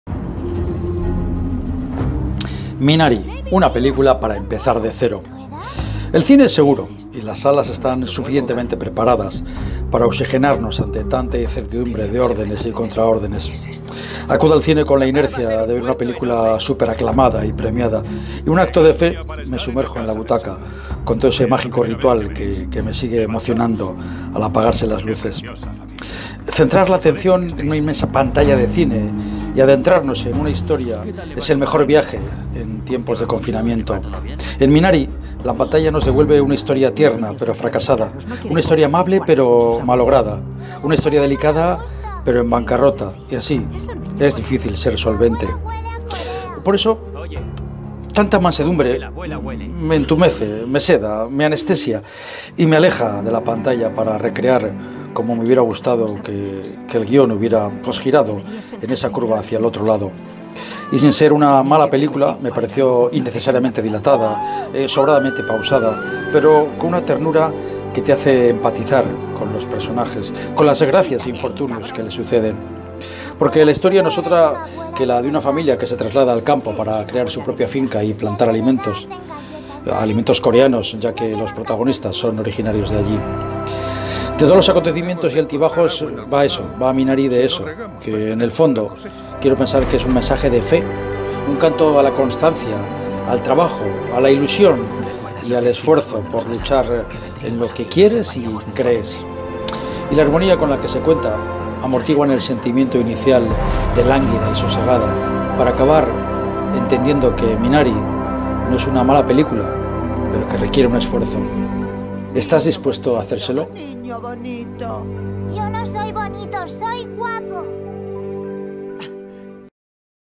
MINARI Cine
MINARIA una película tierna
El cine es seguro y las salas están suficientemente preparadas para oxigenarnos ante tanta incertidumbre de órdenes y contraordenes.
Acudo al cine con la inercia de ver una película súper aclamada y premiada, y en un acto de fe, me sumerjo en la butaca con todo ese mágico ritual que me sigue emocionando al apagarse las luces.
Centrar la atención en una inmensa pantalla de cine y adentrarnos en una historia, es el mejor viaje en tiempos de confinamiento.
En Minari, la pantalla nos devuelve una historia tierna pero fracasada. Una historia amable pero malograda, Una historia delicada pero en bancarrota y así es difícil ser solvente.
Por eso tanta mansedumbre me entumece, me seda, me anestesia y me aleja de la pantalla para recrear como me hubiera gustado que el guion hubiera girado en esa curva hacia el otro lado. Y sin ser una mala película, me pareció innecesariamente dilatada, sobradamente pausada pero con una ternura que te hace empatizar con los personajes, con las desgracias e infortunios que les suceden.
Porque la historia no es otra que la de una familia que se traslada al campo para crear su propia finca y plantar alimentos Coreanos, ya que los protagonistas son originarios de allí.
De todos los acontecimientos y altibajos va Minari, que en el fondo quiero pensar que es un mensaje de FE, un canto a la constancia, al trabajo, a la ilusión y el esfuerzo por luchar en lo que quieres y crees. Y la armonía con la que se cuenta, amortiguan el sentimiento inicial de lánguida y sosegada, para acabar entendiendo que Minari, no es una mala película pero requiere un esfuerzo.
¿Estás dispuesto hacerle?

Jabier Calle – Bilbao 6 Marzo DOSMIL21